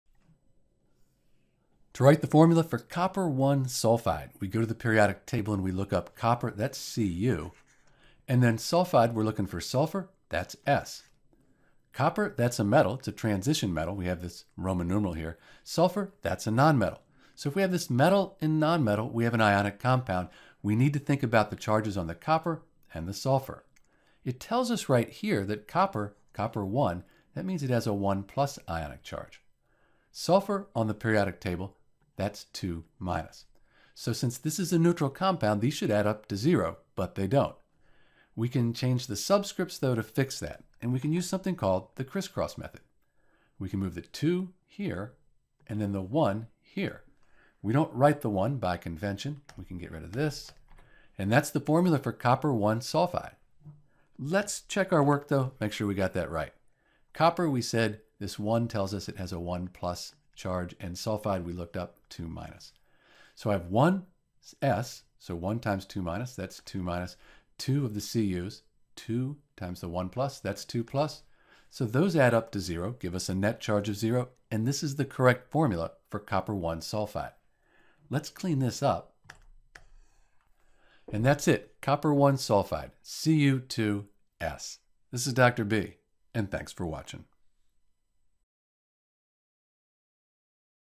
How to Write the Formula for Copper (I) sulfide
In this video we'll write the correct formula for Copper (I) sulfide, Cu2S.

To write the formula for Copper (I) sulfide we’ll use the Periodic Table and follow some simple rules.

1. Write the symbol and charge for the transition metal. The charge is the Roman Numeral in parentheses.
2. Write the symbol and charge for the non-metal. If you have a polyatomic ion, use the Common Ion table to find the formula and charge.
3. See if the charges are balanced (if they are you’re done!)
4. Add subscripts (if necessary) so the charge for the entire compound is zero.
5. Use the crisscross method to check your work.

For a complete tutorial on naming and formula writing for compounds, like Copper (I) sulfide and more, visit: